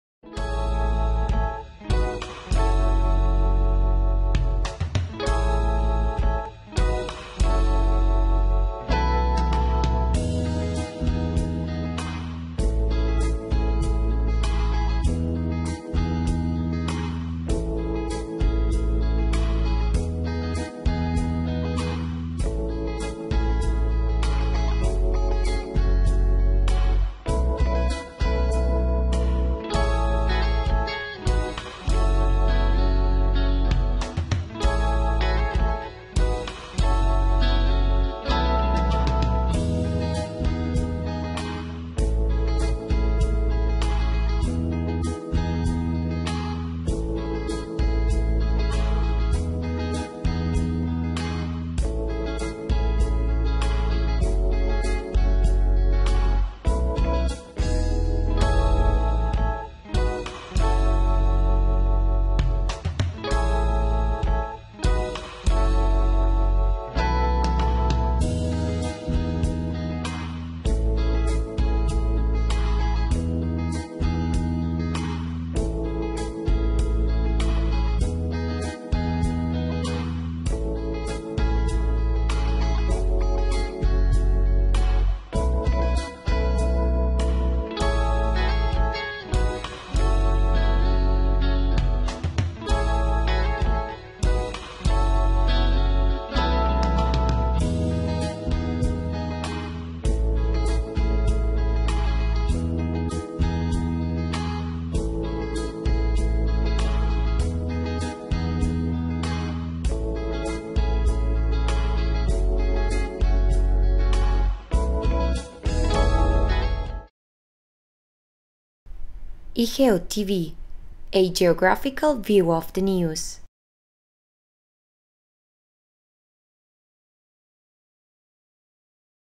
Un año de circuitos Formula 1 / A year of F1 circuits [IGEOTV]
La temporada 2013 de Fórmula 1 es el 64.º campeonato mundial de Fórmula 1 de la historia. Durante la misma, continúa vigente el Acuerdo de la Concordia. La temporada comenzó el 17 de marzo con el GP de Australia y finalizará el 24 de noviembre en Brasil. Once escuderías y veintidós pilotos compiten por ser campeones. Sebastian Vettel y Red Bull Racing son los defensores del título de campeón de pilotos y de constructores, respectivamente.

The 2013 Formula 1 season is 64. º Formula 1 world championship in history. During that period, remains in effect the Concorde Agreement. The season began on March 17 with the GP of Australia and will end on November 24 in Brazil. Eleven teams and twenty riders competing to be champions. Sebastian Vettel and Red Bull Racing are the defending champions of champion drivers and constructors, respectively.